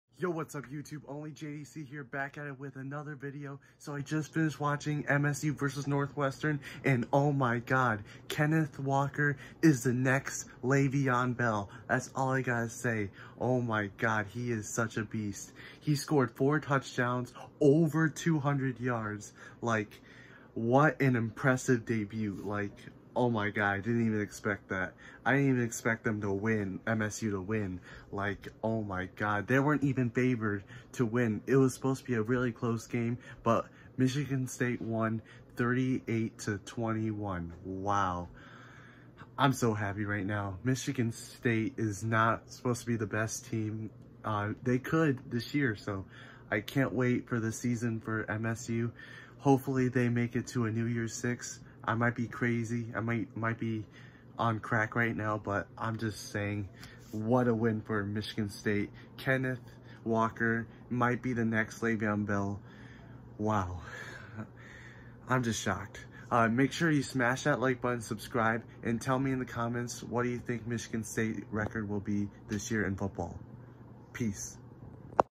IS KENNETH WALKER THE NEXT LE’VEON BELL? MICHIGAN STATE BIG WIN!!!
Michigan upset against northwestern!!!